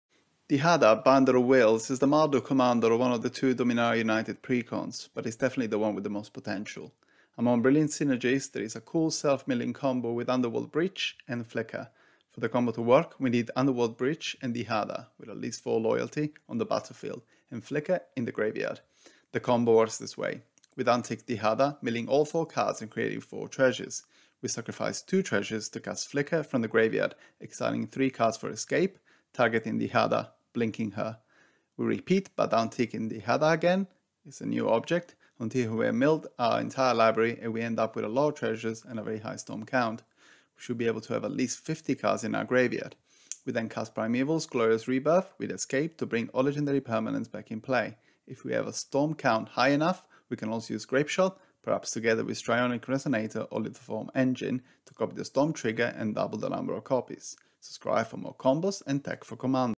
Commander Combos - NEW Dihada, Binder of Wills with Underworld Breach and Flicker
Everyone likes combos with the Commander, so here's one with Dihada and some graveyard recursion!